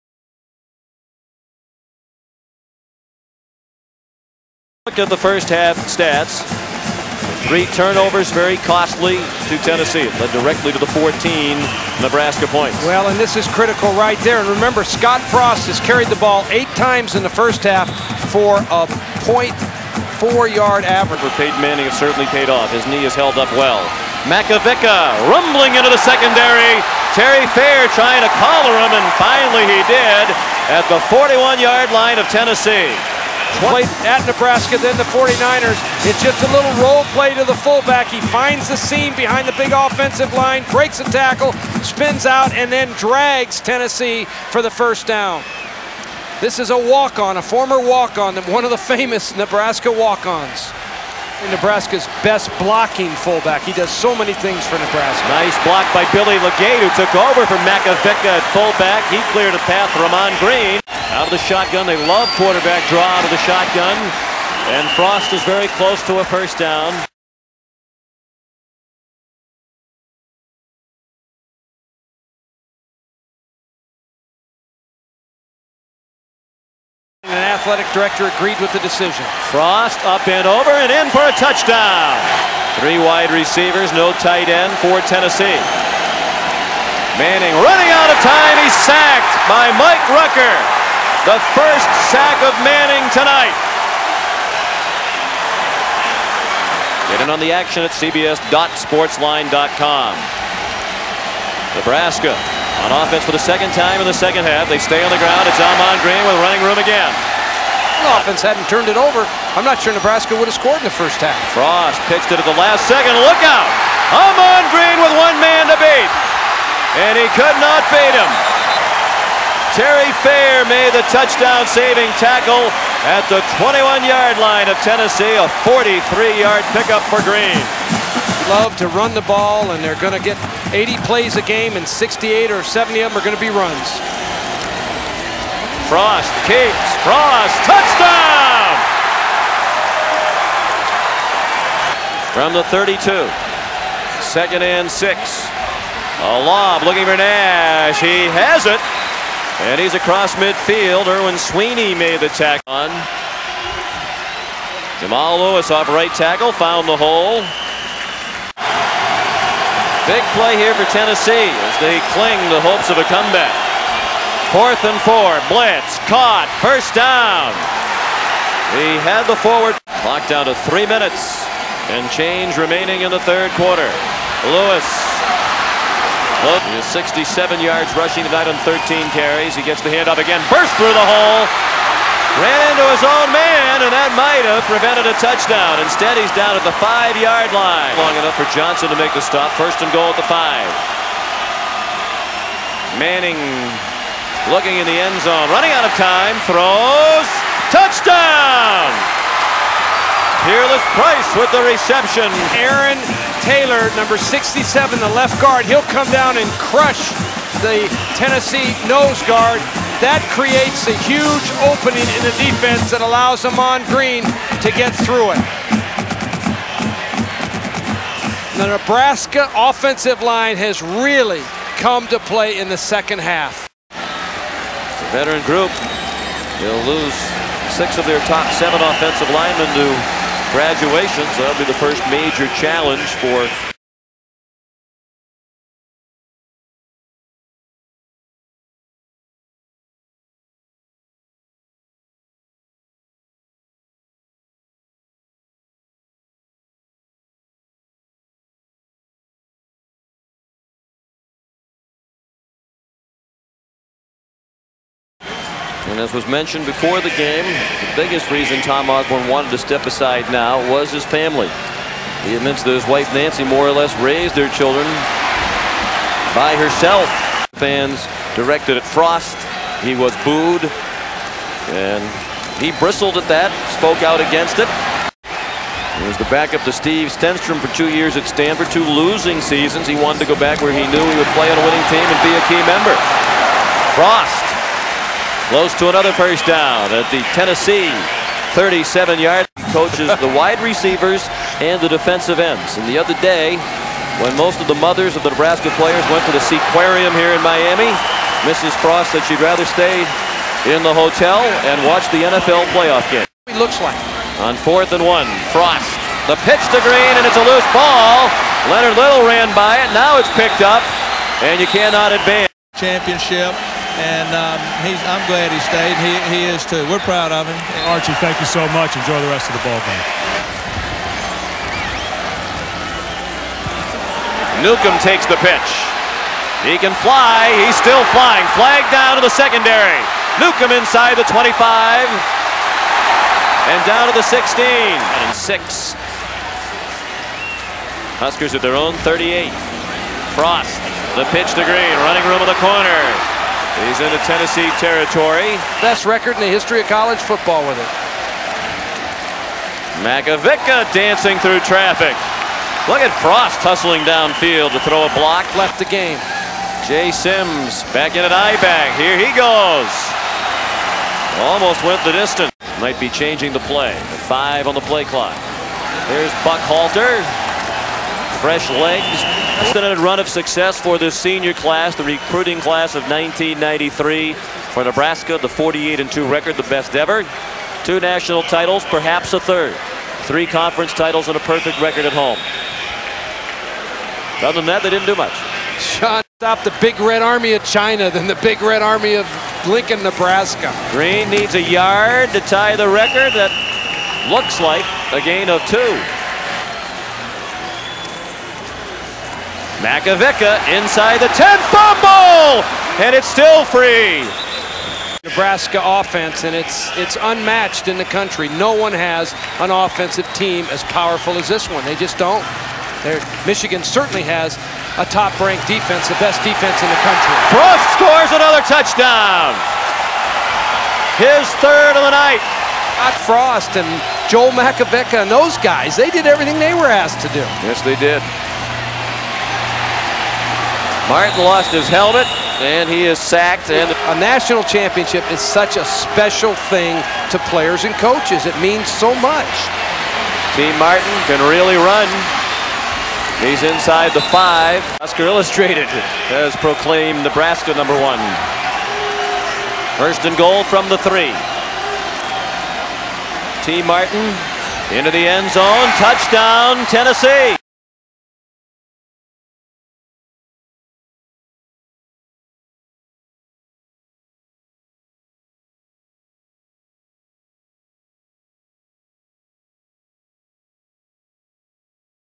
Part 2 1997 Orange Bowl Nebraska vs Tennesse
The second half of this game, Nebraska piles up 335 rushing yards in the second half to roll #3 Tennesse and claim a share of the 1997 MNC.  Ahman Green goes for 206 yards in his final game as a Husker. The final collegiate game for Tom Osborne,Ahman Green, Scott Frost, Peyton Manning, Grant Wistrom, Jason Peter.